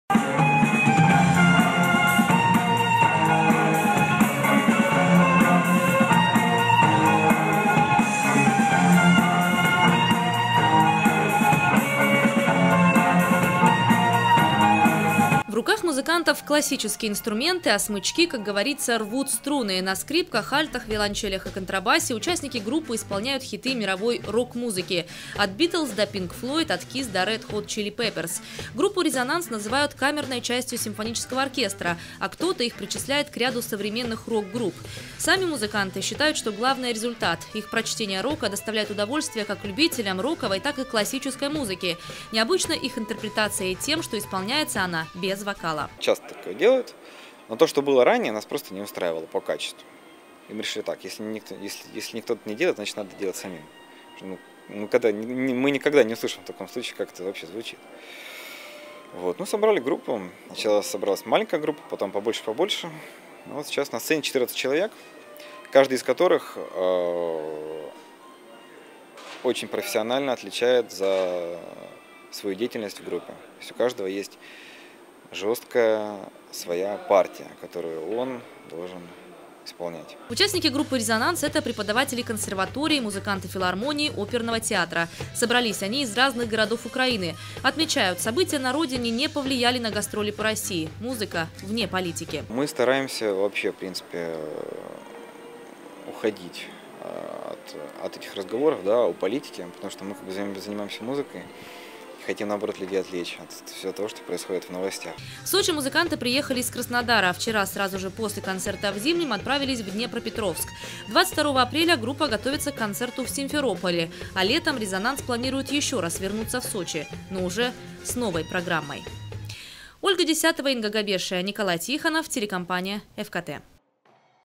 В Сочи выступила группа "Резонанс". Новости 24 Сочи
Свободных мест в Зимнем театре, не было. Сочинские ценители классики рока, все билеты раскупили заранее. Музыканты предлагают свое прочтение роковых произведений с помощью классических струнных инструментов.
Сочи. Новости.
Телеканал Эфкате.